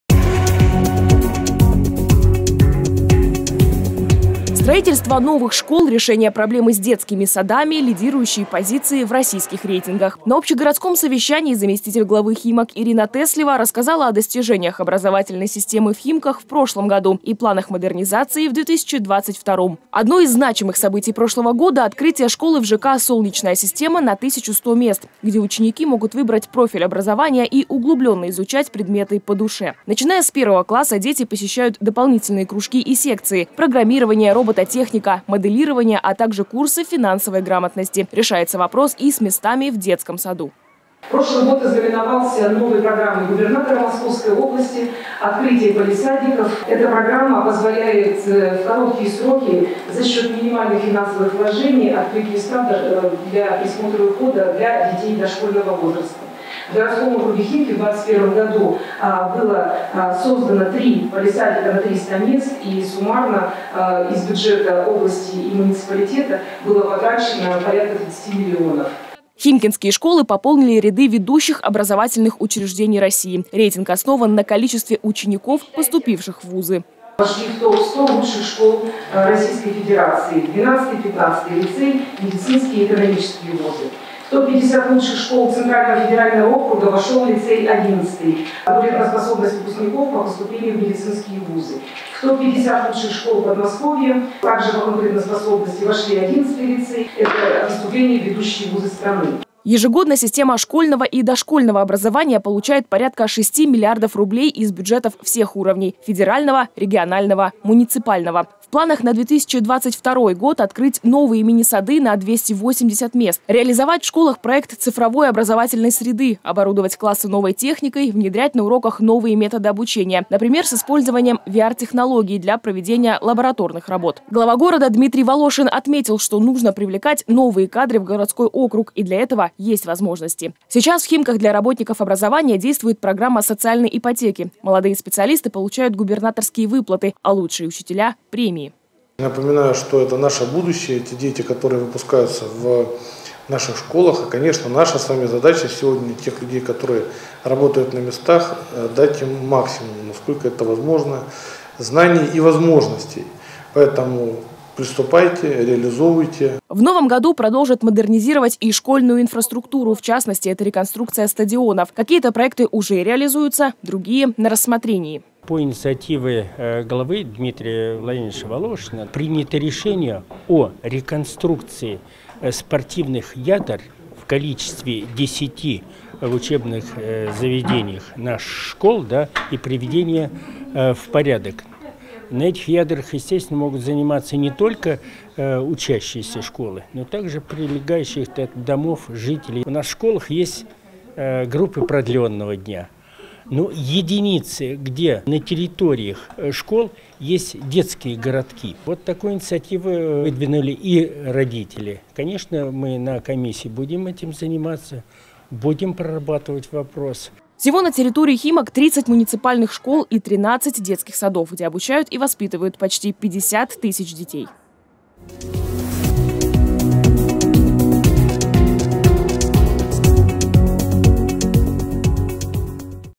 Химкинские школы в рейтинге лучших в России. 20.01.2022
Строительство новых школ, решение проблемы с детскими садами, лидирующие позиции в российских рейтингах. На общегородском совещании заместитель главы Химок Ирина Теслева рассказала о достижениях образовательной системы в Химках в прошлом году и планах модернизации в 2022.